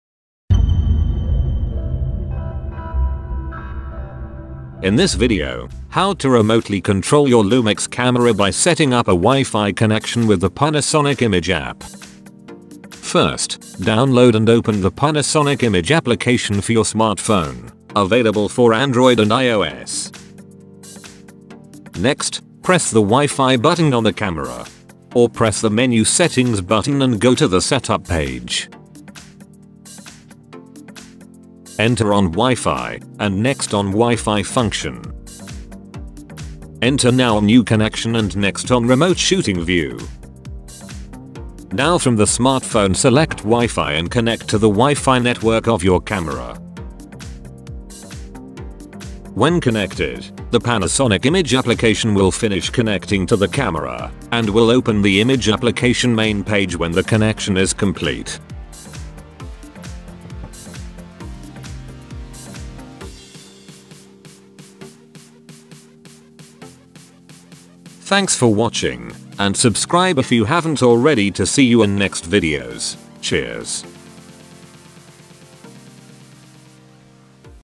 Panasonic Lumix Image App WiFi Set-Up (Remote Shooting View)
Panasonic Lumix Remote Shooting View. How to remotely control your camera by setting up Wi-Fi connection with Panasonic Image App.

Download and Install the Panasonic Image Application for your smartphone (Android, iOS).

1-  Press the Wi-Fi Button on the camera, or press the menu settings button.
2- Go to the setup page.
3- Enter on wifi, and next on wifi function.
4- Set a new connection.
5- Remote shooting view.
6- Now from the smartphone select wi-fi and connect to the wifi network of your camera.

When connected, the Panasonic Image Application will finish connecting to the camera, and will open the Image Application main page when the connection is complete.